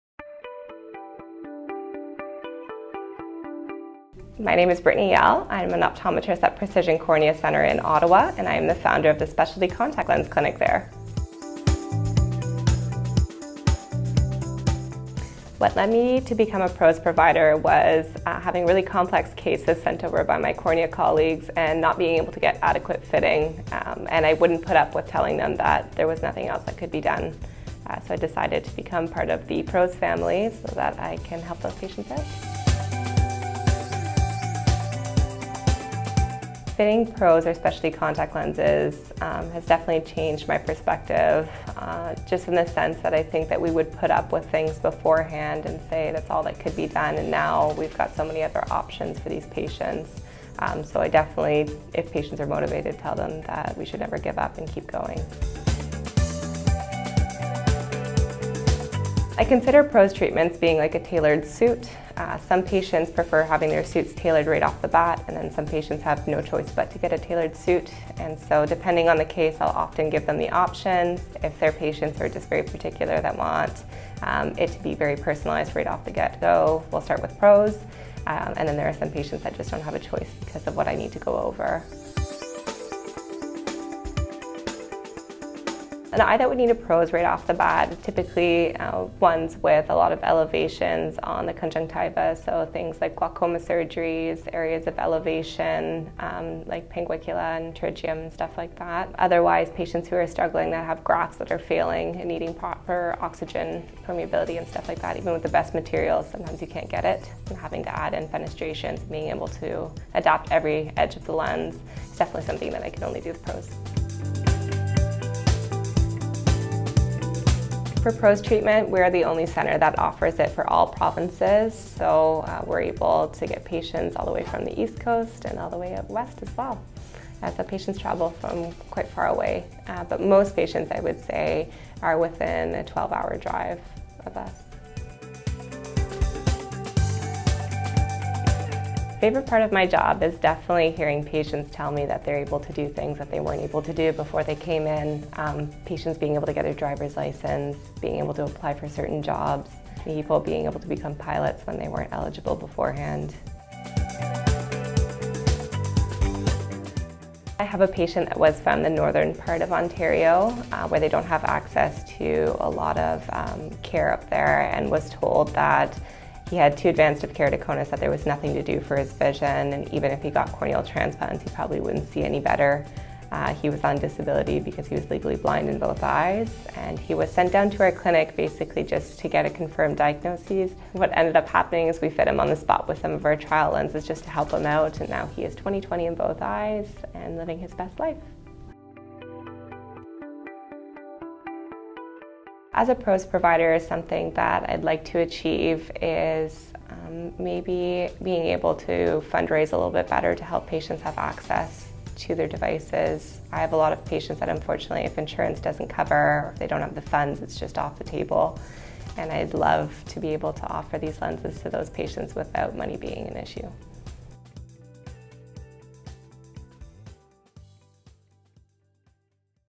PROSE Provider Spotlight: Dr. Brittany Yelle | Precision Cornea Centre | BostonSight PROSE
Learn about Dr. Brittany Yelle from Precision Cornea Centre and her work with PROSE patients. Dr. Yelle discusses what PROSE is, how she uses PROSE with patients, and what to know about PROSE treatment.